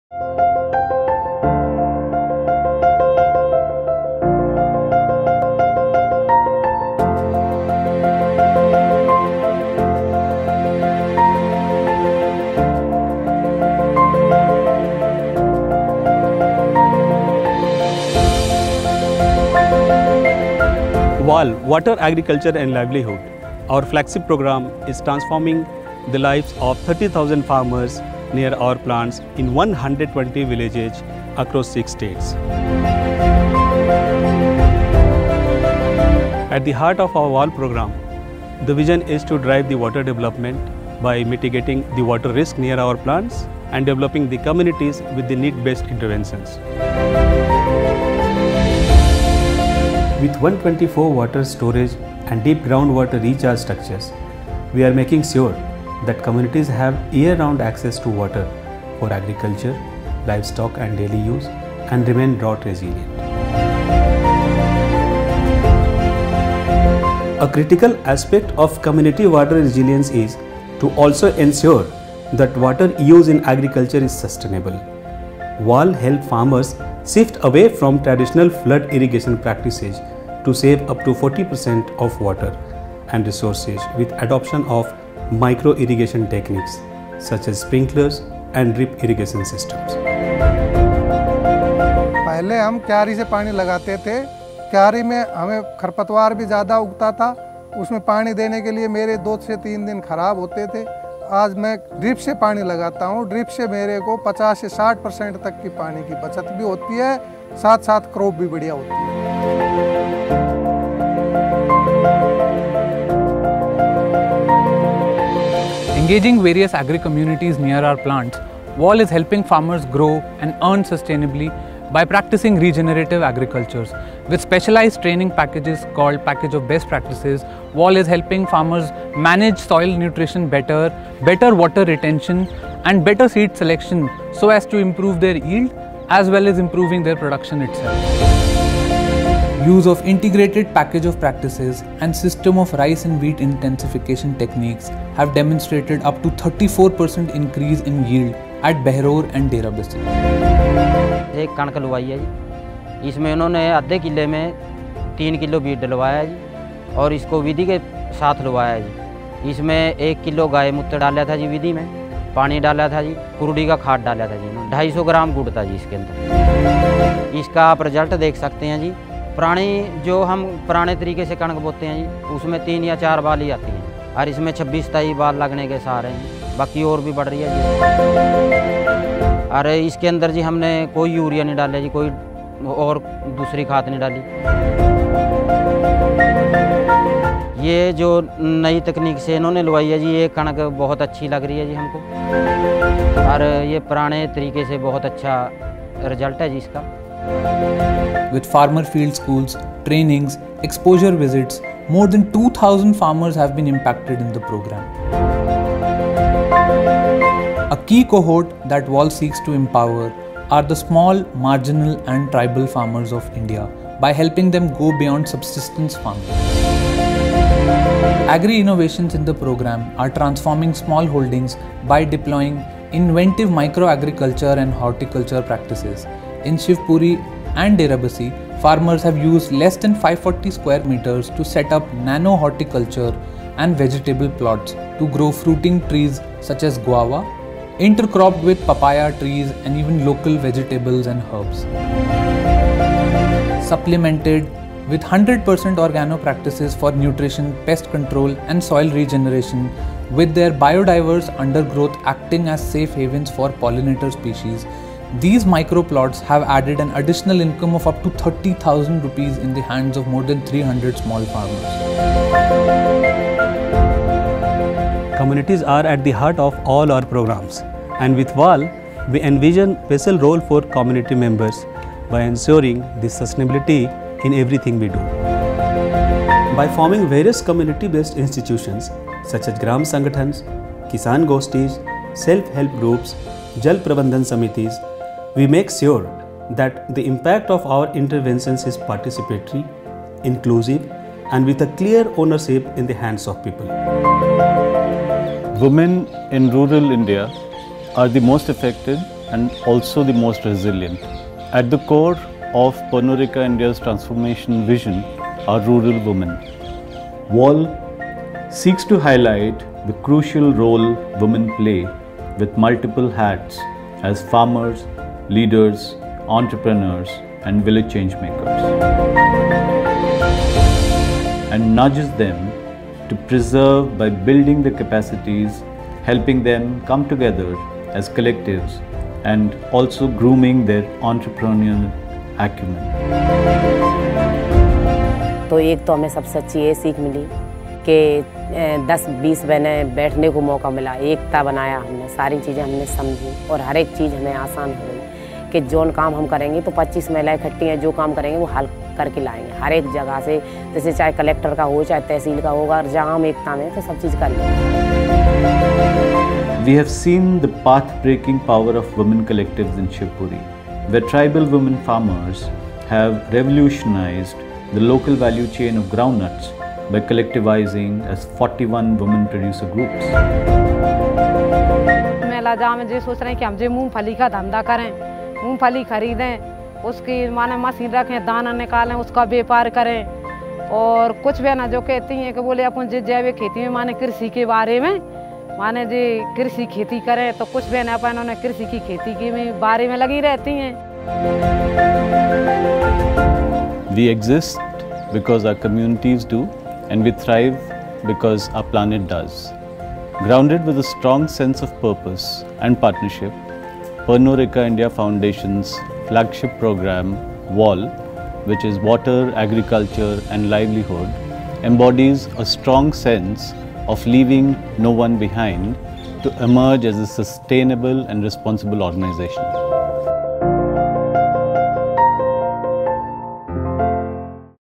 Empowering 30,000 farmers with Water, Agriculture, Livelihoods (WAL)
We exist because our communities do and we thrive because our planet does. Grounded in a strong sense of purpose and partnerships, Pernod Ricard India Foundation's flagship program, WAL (Water, Agriculture & Livelihoods) embodies our vision of LeavingNoOneBehind.

Watch the film to discover how the program is fostering water resilience while promoting sustainable agriculture and enhancing livelihoods of rural communities around our operations in more than 6 states.